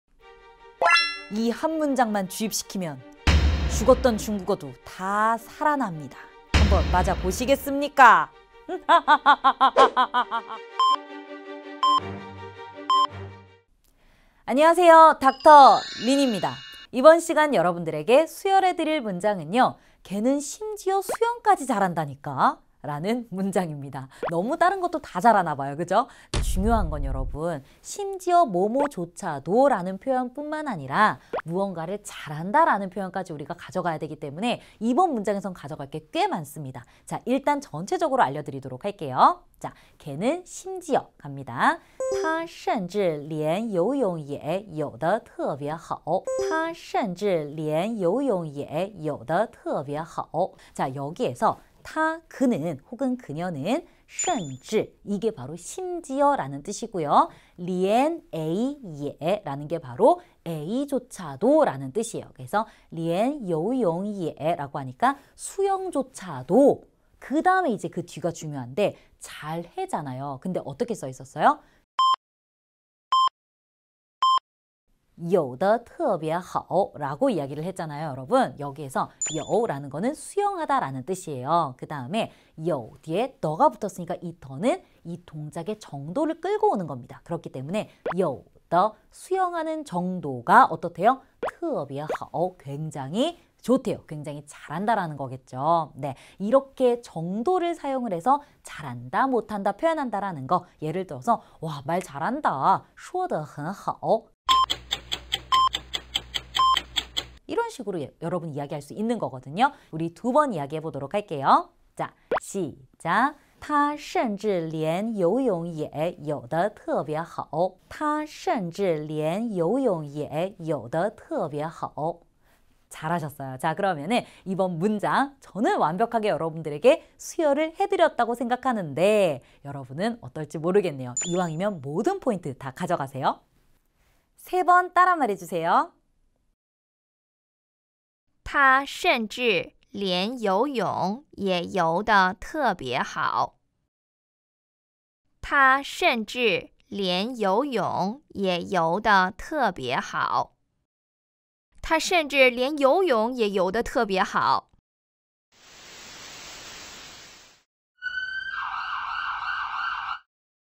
[중국어 한마디] 걔는 심지어 수영까지 잘한다니까! - 운동 편(중국어 회화, 중국어 기초, 중국어 공부, 중국어 단어)
당신의 멈춘 실력을 다시 뛰게한다. 매일매일 꾸준히 긴급구조 119 중국어

[문장]
걔는 심지어 수영까지 잘한다니까!
他(她)甚至连游泳也游得特别好。
Tā (tā) shènzhì lián yóuyǒng yě yóu de tèbié hǎo.

[단어 및 표현]
甚至 shènzhì 심지어, ~까지도
连A也…… lián A yě…… A조차도 ~하다
游泳 yóuyǒng 수영하다
游 yóu 수영하다
得 de 동사, 형용사의 뒤에 붙어, 결과나 정도를 표시하는 보어를 연결시키는 역할을 함
特别 tèbié 특별히, 아주

[Tip]
오늘의 짝꿍 표현은 ‘连’과 ‘也’입니다! ‘连A也……’라고 하면 ‘A조차도 ~하다’라는 표현이 되는데요, 표현 앞에 ‘심지어’라는 뜻의 ‘甚至’까지 붙이면 ‘甚至连A也……’, ‘심지어 A조차도 ~하다’라는 표현이 됩니다!